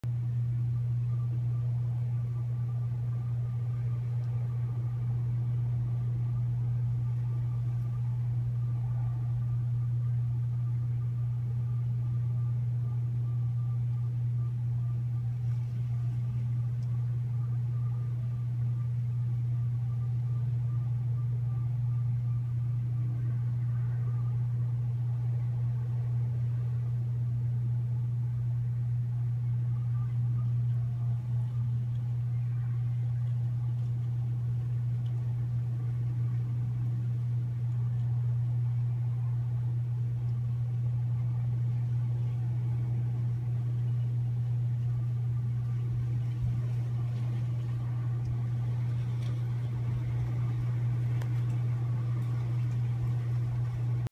I found an ant with another ants decapitated head clamping its antenna.
Ant is trying to remove another ants head from it's antennae.